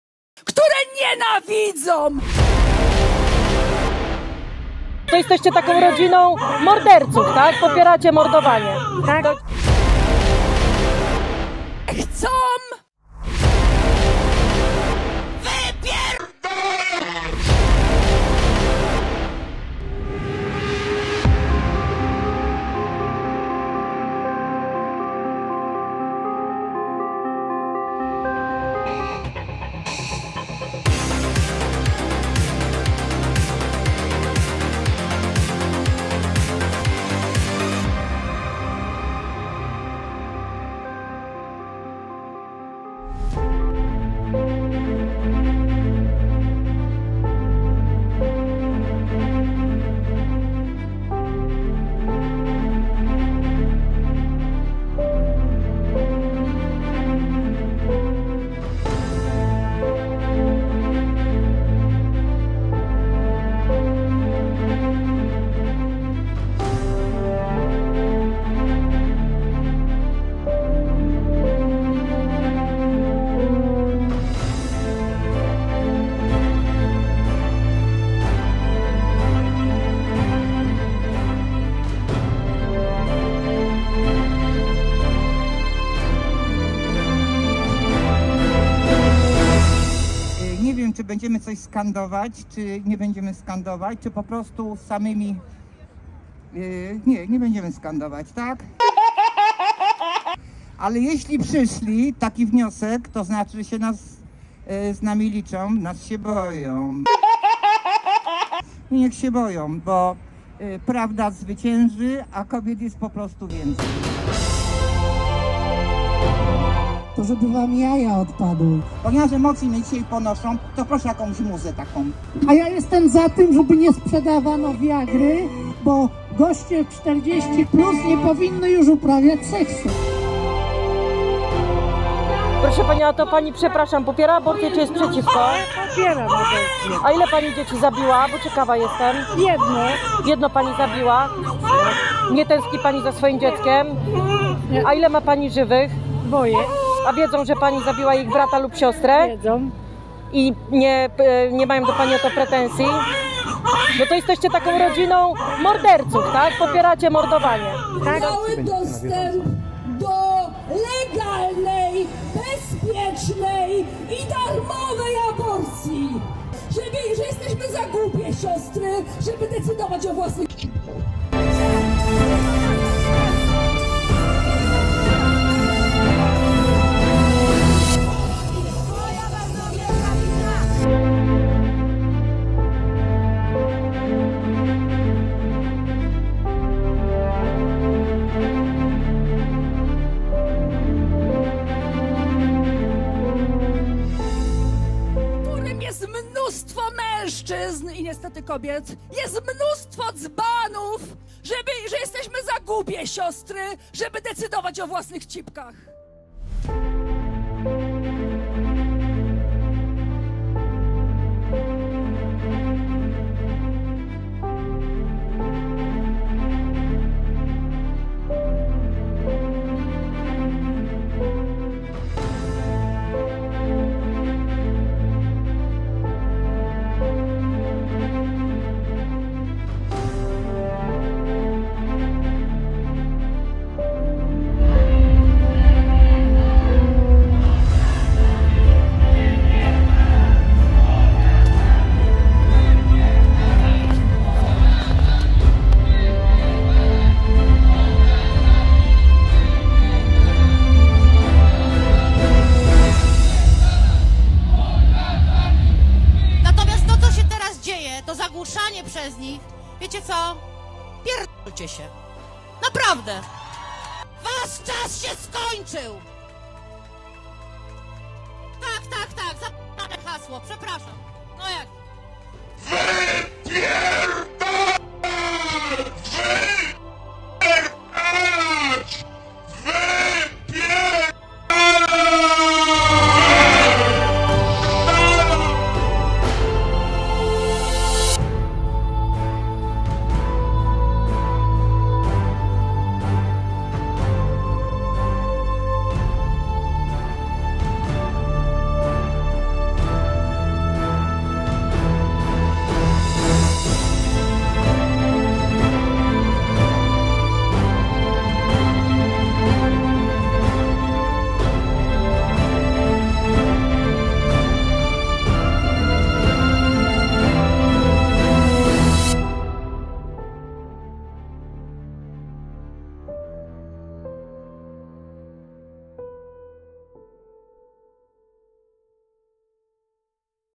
Porażka aborterów. Polska przeciw mordowaniu!
W kilku miastach odbyły się czarne protesty. Wszędzie byliśmy z prawdą o aborcji. Wszędzie spotkaliśmy się z tradycyjną, niecenzuralną "tolerancją" środowisk lewicowych. Zobaczcie sami!

Podoba Ci się to, co robimy?